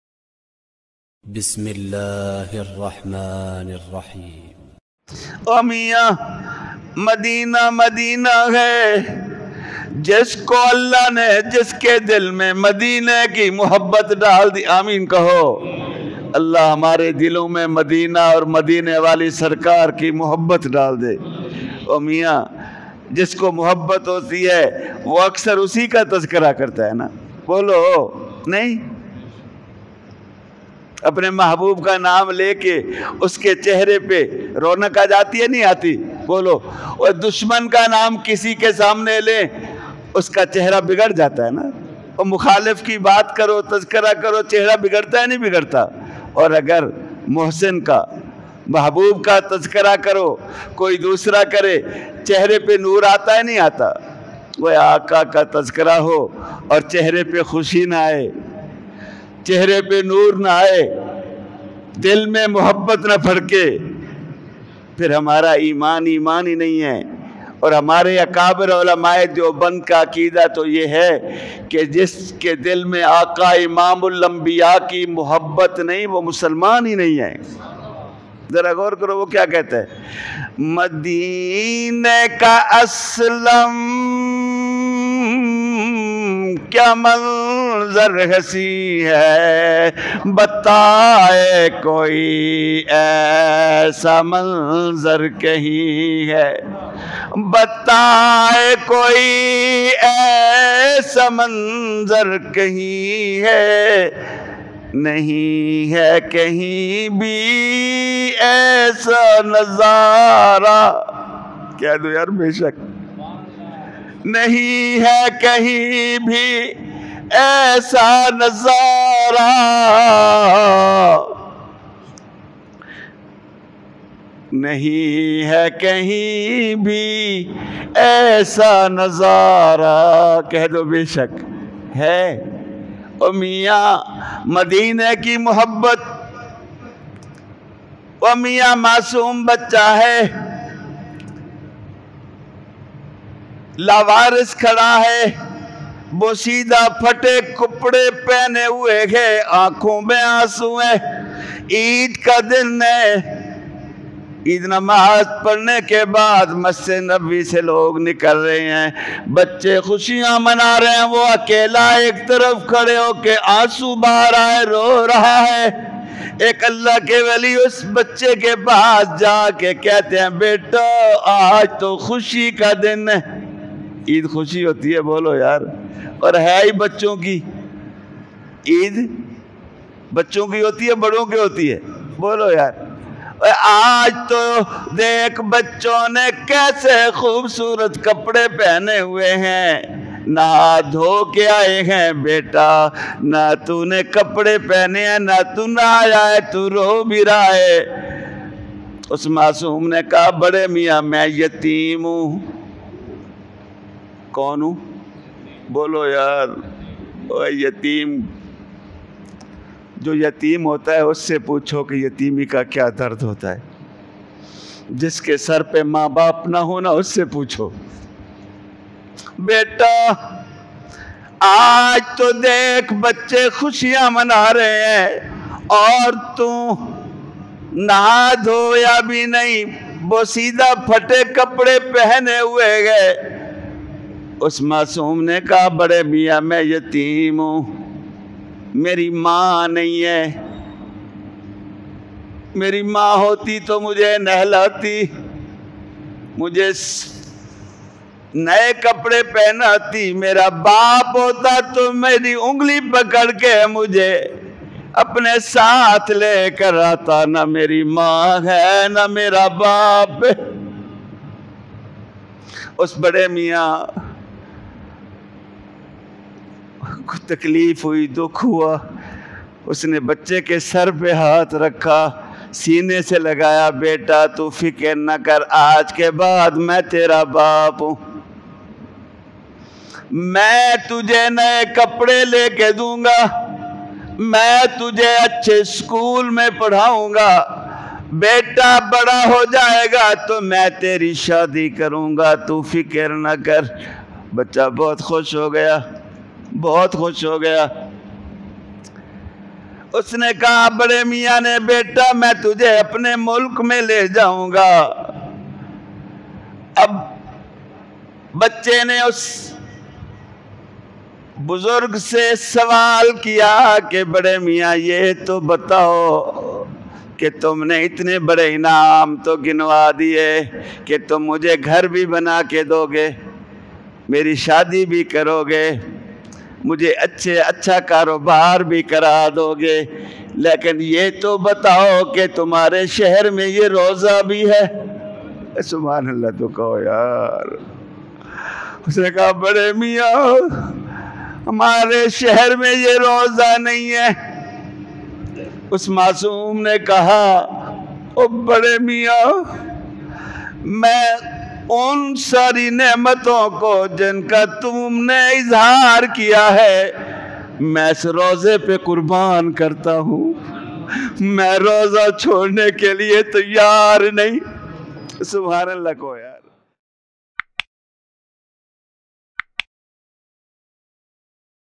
The story of an innocent child's love for the Paighambar | Maulana Fazal Ur Rahman Darkhwasti
ایک یتیم بچے کا حضورﷺ سے عشق کا واقعہ